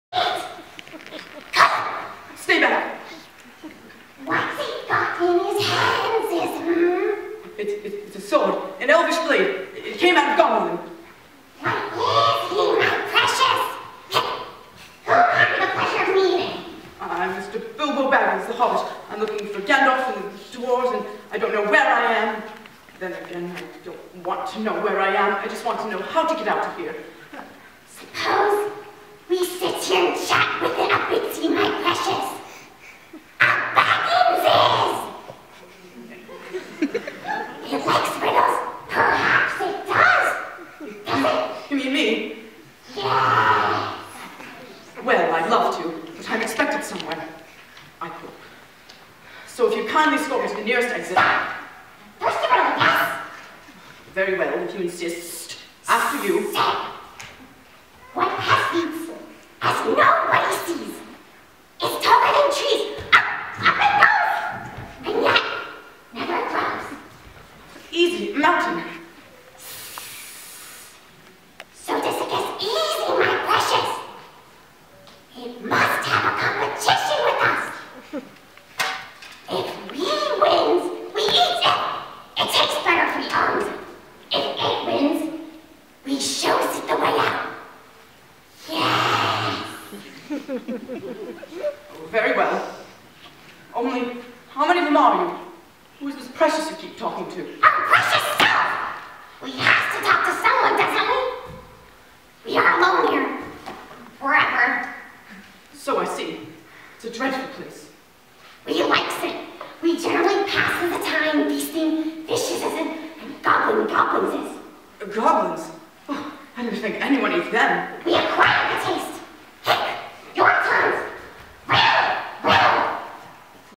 Gollum | The Hobbit | Fairfield Ludlowe High School
Fairfield Ludlowe High School's production of The Hobbit. December 2012.

Roni Mandelkern as Gollum
James Purcell as Bilbo Baggins